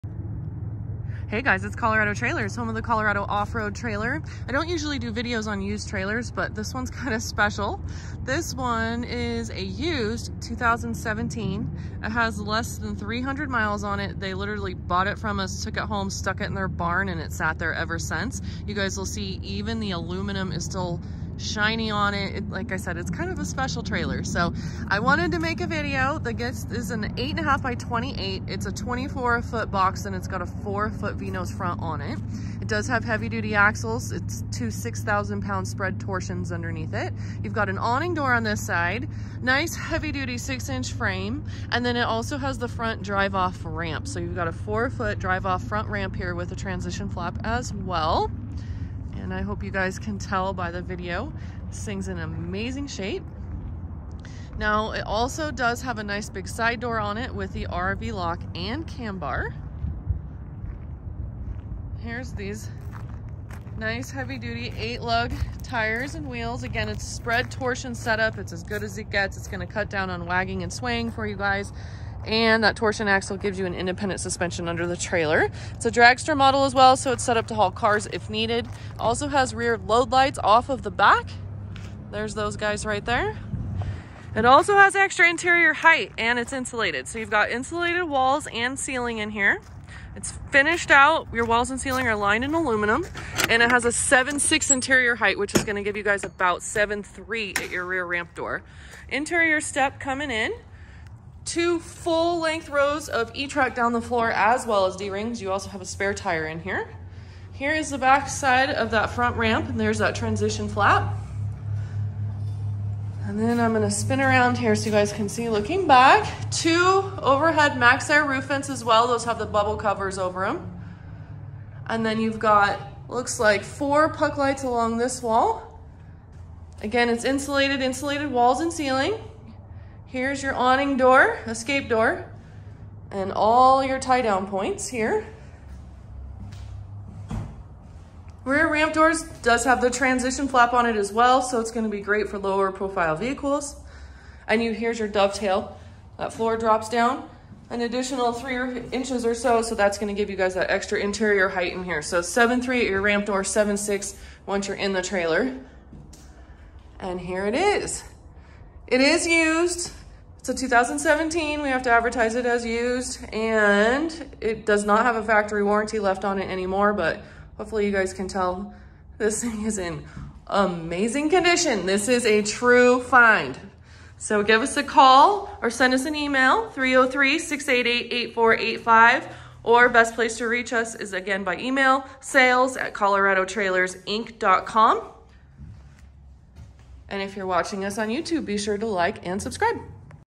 BARN FIND! Used 8.5x28 used cargo trailer for sale
Used 2017 8.5x28 Dragster Cargo Trailer for sale.  Trailer features:

2 - 6000lb. spread torsion axles with electric trailer brakes
7K Drop Leg Jack
7 foot 6 inch interior height
24 foot floor with 4 foot V nose front
5x6 side escape/awning door
4 foot drive off front ramp with transition flap
Rear ramp door with spring assist close, transition flap and cam bars
Side door with RV lock and cam bar
4 D-rings in floor
E-Track in floor
Insulated walls and ceiling
Aluminum wall and ceiling liner
2 roof vents with Maxx Air and A/C braces
Painted interior floor
Trailer is sold as is no warranty
Trailer is in EXCELLENT condition and has less than 300 miles on it and was stored in doors!

Year: 2017
Make: Cargo Craft
Model: 8.5x28
Color: White

Shipping options available with great rates! Call to inquire.